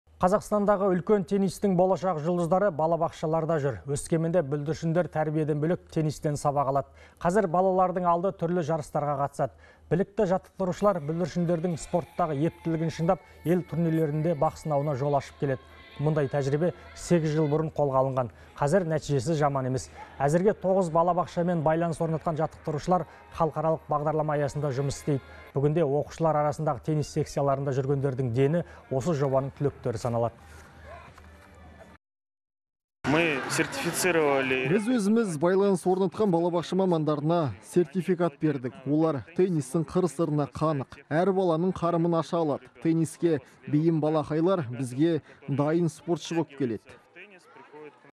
Өскеменде балабақшадағы бүлдіршіндер теннистен жарысқа қатысып жүр
Қазақстандағы үлкен теннистің болашақ жұлдыздары балабақшаларда жүр.

Өскеменде бүлдіршіндер тәрбиеден бөлек теннистен сабақ алады. Қазір балалардың алды түрлі жарыстарға қатысады. Білікті жаттықтырушылар бүлдіршіндердің спорттағы ептілігін шыңдап, ел турнирлерінде бақ сынауына жол ашып келеді. Мұндай тәжірибе 8 жыл бұрын қолға алынған. Қазір нәтижесі жаман емес. Әзірге 9 балабақшамен байланыс орнатқан жаттықтырушылар халықаралық бағдарлама аясында жұмыс істейді. Бүгінде оқушылар арасындағы теннис секциясында жүргендердің дені осы жобаның түлектері саналады.

Евгений Нефедов, жаттықтырушы:

- Біз өзіміз байланыс орнатқан балабақша мамандарына сертификат бердік. Олар теннистің қыр-сырына қанық. Әр баланың қарымын аша алады. Тенниске бейім балақайлар бізге дайын спортшы болып келеді.